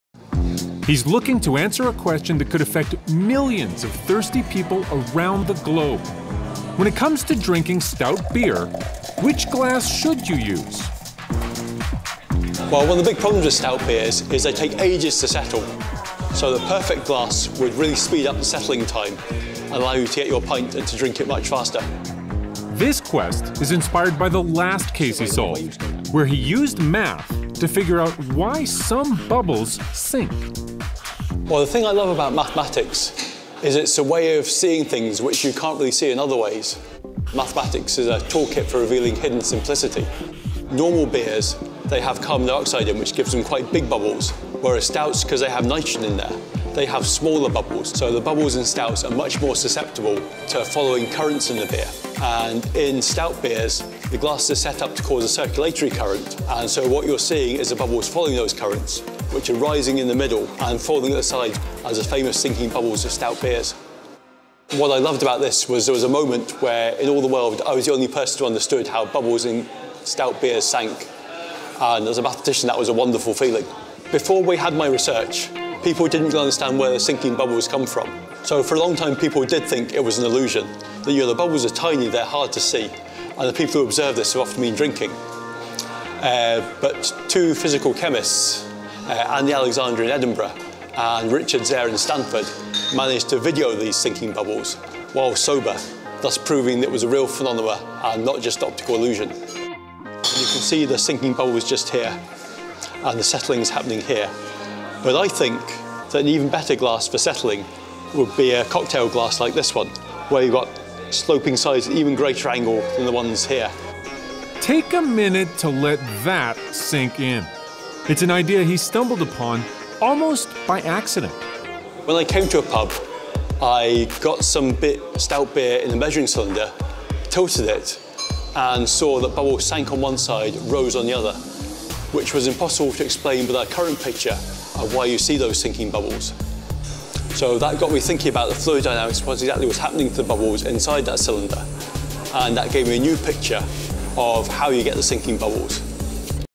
The best way to drink Guinness is actually from a cocktail glass!
A mathematical equation which demonstrates how a pint of Guinness should be poured in order to make the most of the bubble's 'settling effect' has been created, tried and tested by the University's Professor of Industrial Mathematics William Lee.  Professor Lee had previously conducted research into the bubbles that appear to move downwards when a pint of Guinness or Stout is initially poured and this caught the eye of the Canadian science show The Daily Planet, which is aired on Canada's Discovery Channel.

They visited Professor Lee in Huddersfield to find out more about the conclusion to his research which is, that Guinness would be best enjoyed in a cocktail-shaped glass and not the familiar tall pint glass we see today.